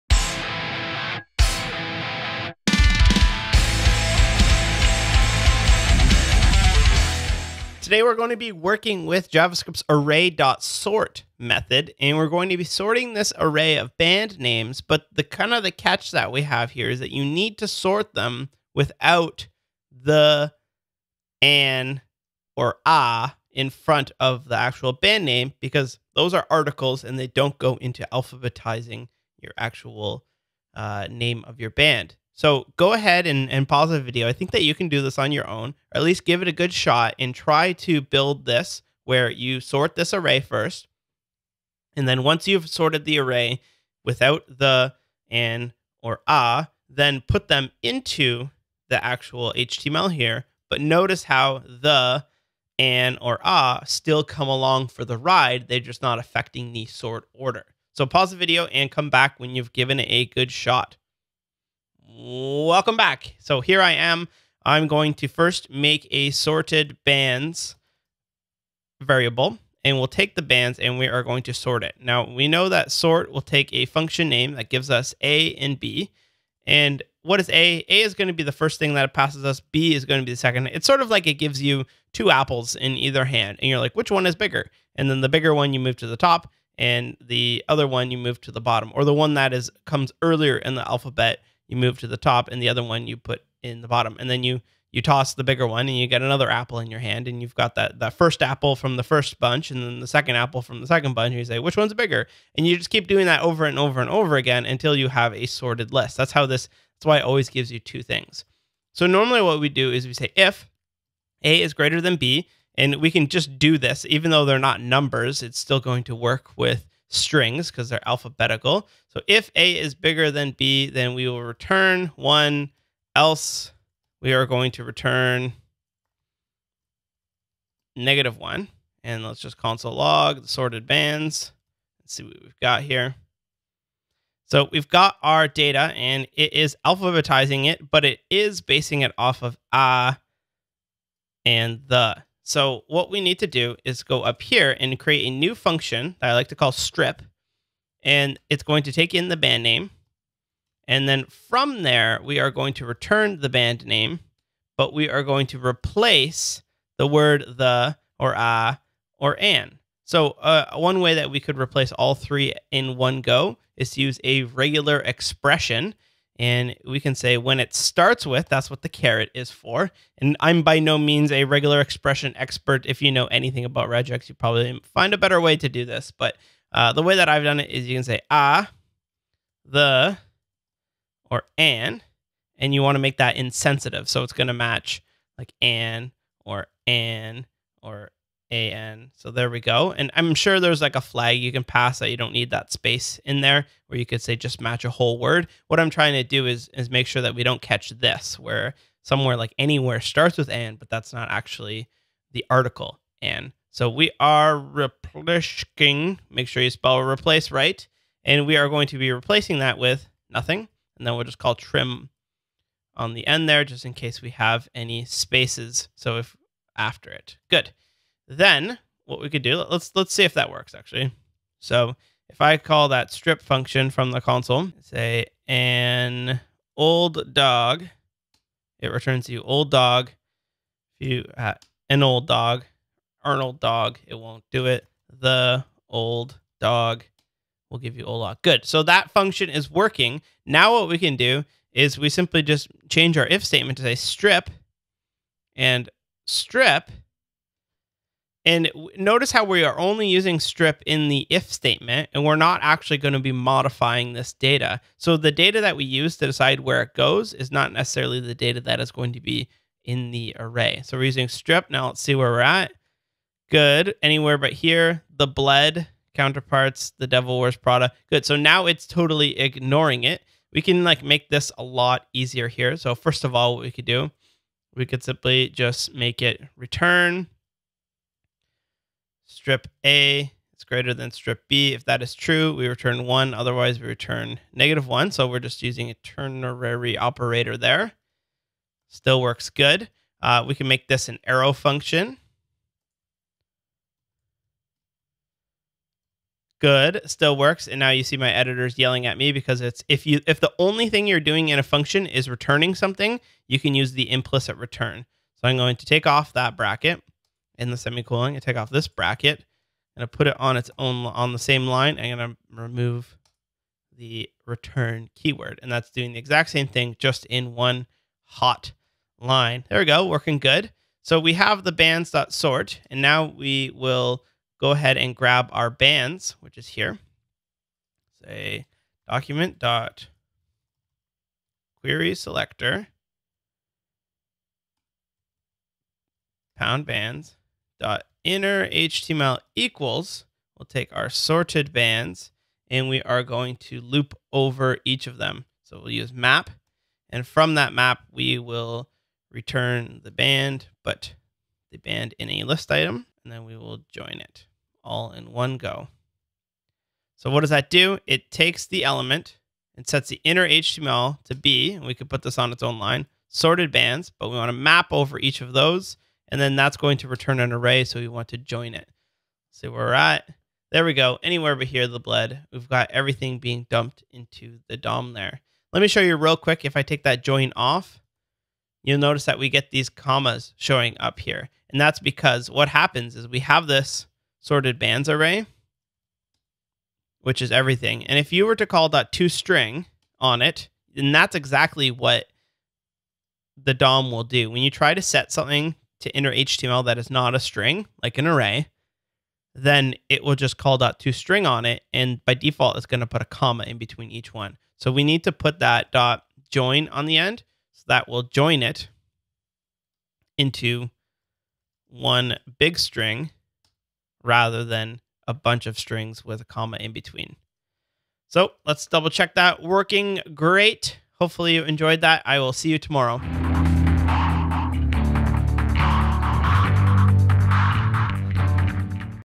JavaScript Practice: Sorting Band Names without articles - JavaScript30 17/30
Today we learn how to sort HTML elements based based on their text content. Grab all the exercises and starter files over